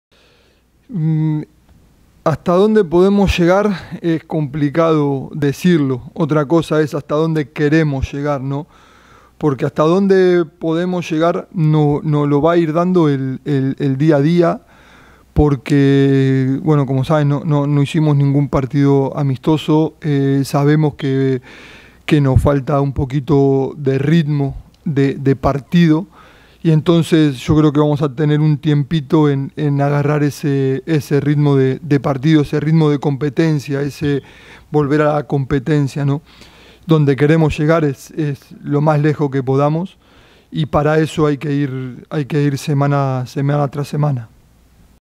Pablo Guede habla sobre hasta donde llegar con Xolos
Pablo Guede habla sobre hasta donde le gustaria llegar con Xolos